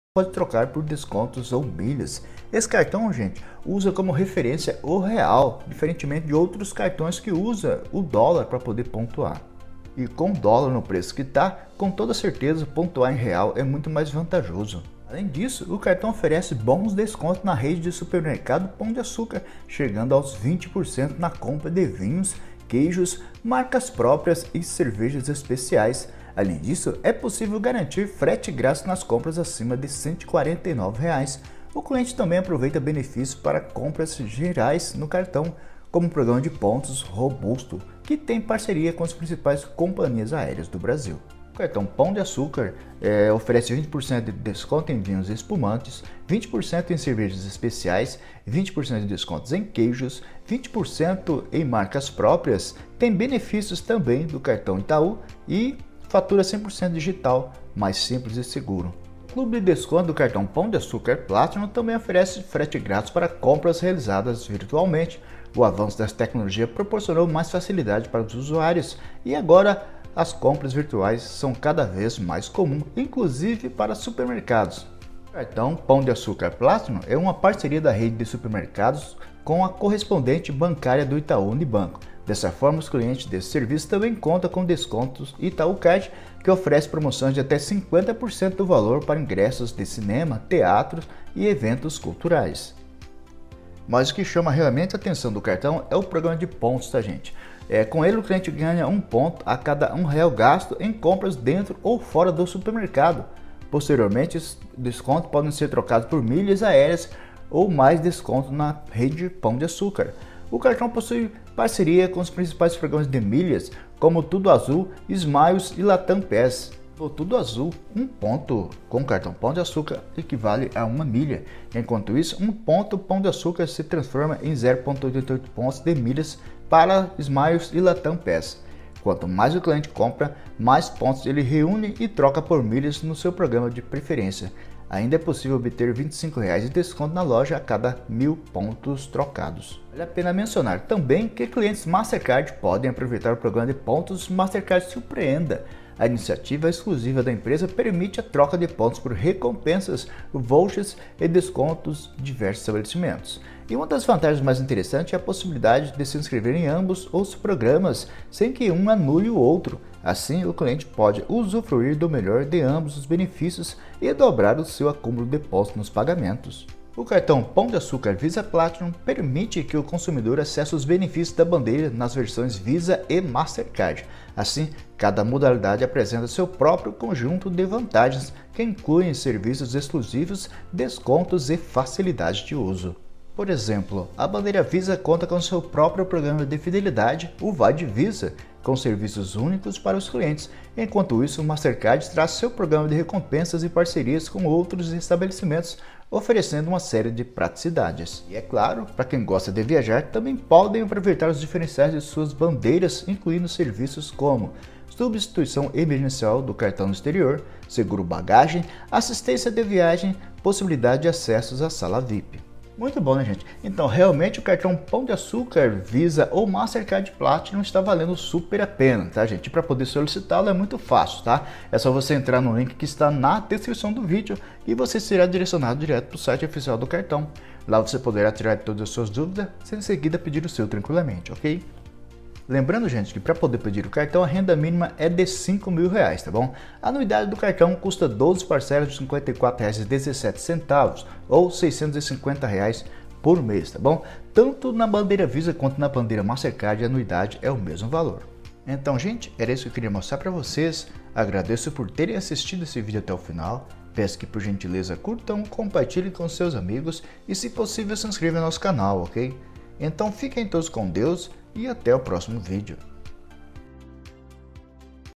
CARTÃO PÃO DE AÇÚCAR PLATINUM: DO MERCADO AO ACÚMULO DE PONTOS, UMA BOA OPÇÃO
O cartão oferece bons descontos na rede de supermercados Pão de Açúcar, chegando aos 20% na compra de vinhos, queijos, marcas próprias e cervejas especiais. Além disso, é possível garantir frete grátis nas compras acima de R$ 149. O cliente também aproveita benefícios para compras gerais no cartão, como um programa de pontos robusto, que tem parceria com as principais companhias aéreas do Brasil.